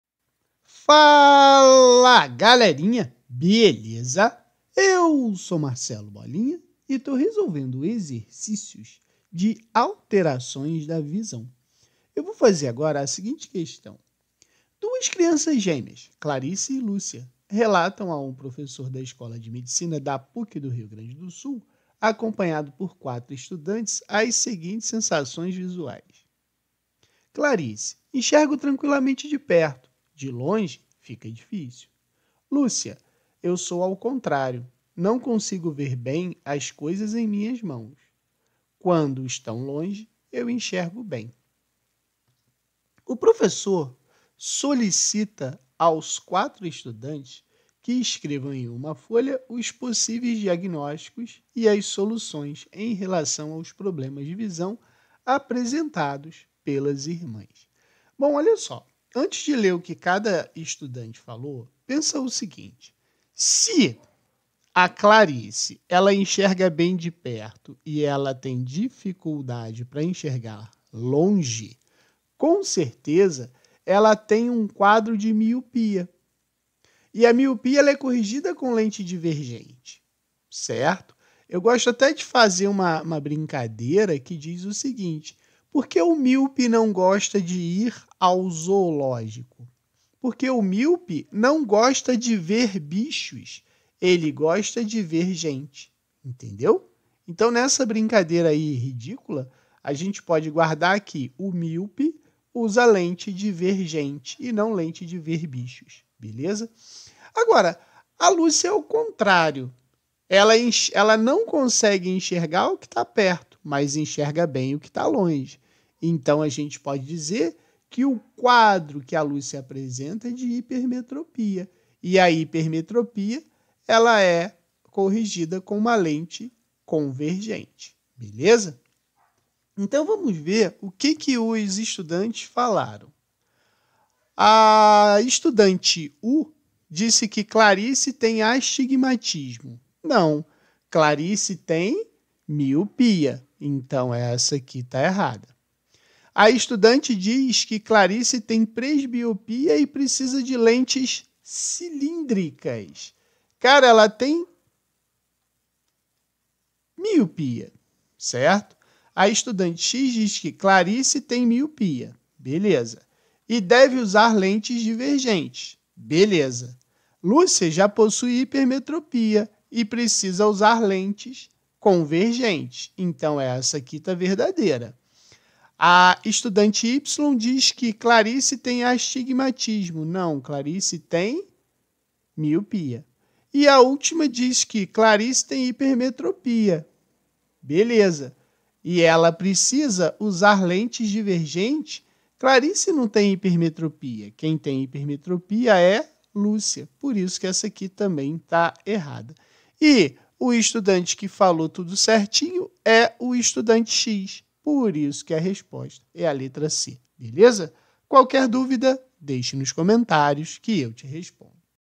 Alterações da visão AS02 Duas crianças gêmeas, Clarice e Lúcia, relatam a um professor
Questão 02 (PUC-RS-Adaptada) Duas crianças gêmeas, Clarice e Lúcia, relatam a um professor da Escola de Medicina da PUC-RS, acompanhado por quatro estudantes, as seguintes sensações visuais:

Clarice: Enxergo tranquilamente de perto. De longe, fica difícil.

Lúcia: Eu sou ao contrário. Não consigo ver bem as coisas em minhas mãos. Quando estão longe, eu enxergo bem.

O professor solicita aos quatro estudantes que escrevam em uma folha os possíveis diagnósticos e as soluções em relação aos problemas de visão apresentados pelas irmãs.

Estudante U: Clarice tem astigmatismo e precisa de lentes cilíndricas. Lúcia tem miopia e precisa de lentes divergentes.

Estudante W: Clarice tem presbiopia e precisa de lentes cilíndricas. Lúcia tem astigmatismo e precisa de lentes divergentes.

Estudante X: Clarice tem miopia e precisa usar lentes divergentes. Lúcia possui hipermetropia e precisa de lentes convergentes.

Estudante Y: Clarice tem astigmatismo e deve usar lentes divergentes. Lúcia apresenta hipermetropia e precisa de lentes cilíndricas.

Estudante Z: Clarice tem hipermetropia e precisa de lentes divergentes. Lúcia possui miopia e deve usar lentes convergentes.

Qual foi o estudante que apresentou o diagnóstico e a solução corretos?

U

W

X

Y

Z